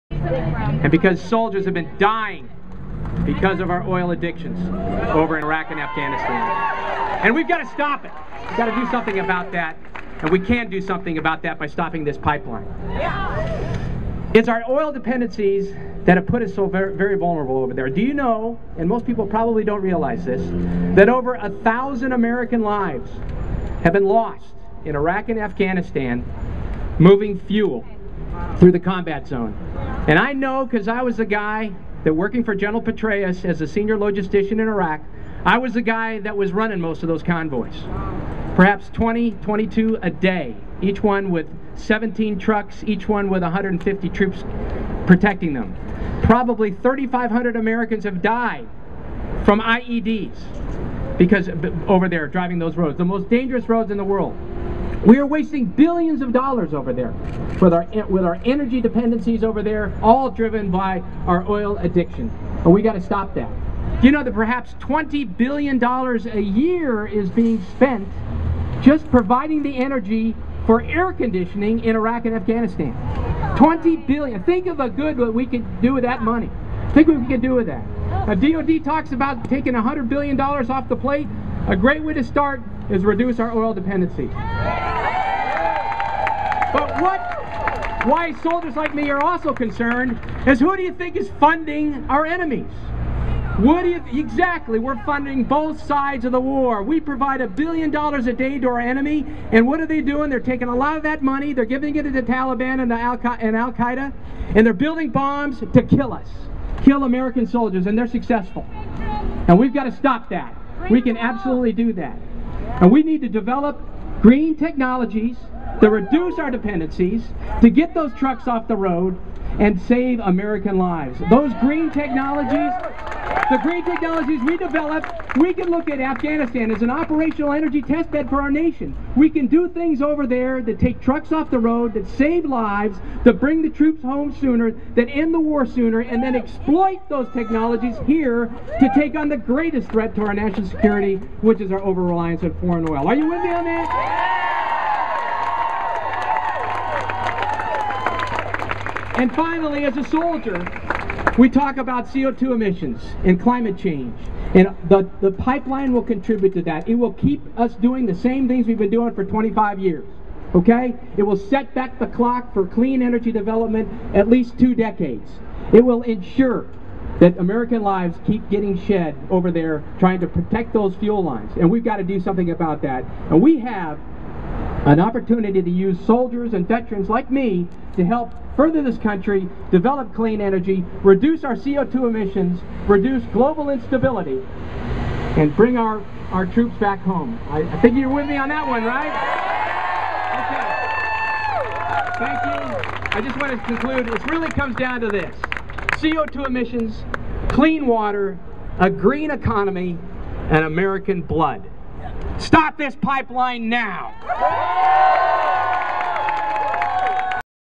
Former High Ranking Officer on Defense and KXL - at Tar Sands Rally at State Dept Hearing, DC
held a rally outside State Department's Tar Sands Hearing in Washington DC on October 7, 2011.  Bill McKibben of 350.org and Mike Tidwell of were among the organizers and speakers.  A number of people from the Climate Change Initiative of Howard County where enthusiatic participants.

106_0163.MOV